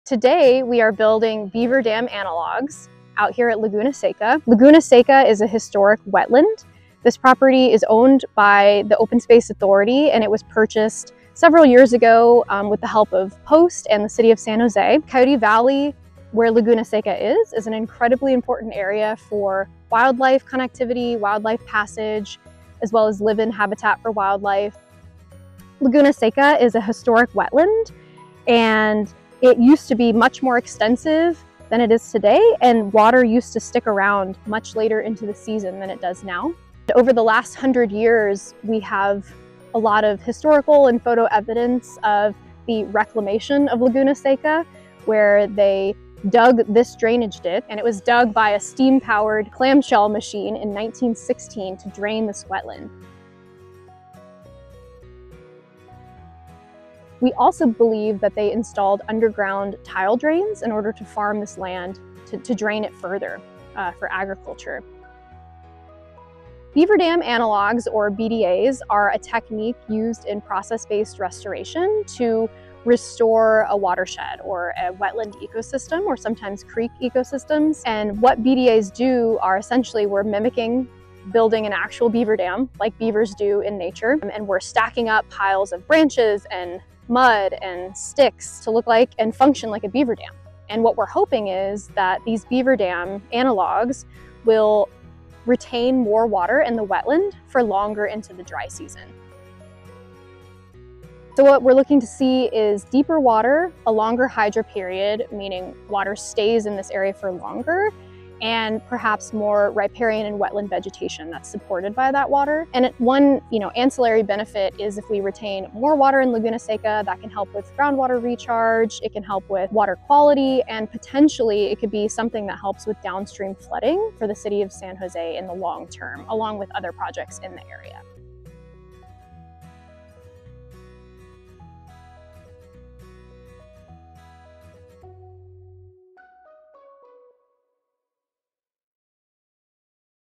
Dam Good News for Laguna Seca
The Open Space Authority worked with the Peninsula Open Space Trust (POST), Alnus Ecological and Nature's Engineers to build two human-made dams, known as beaver dam analogs, in Laguna Seca.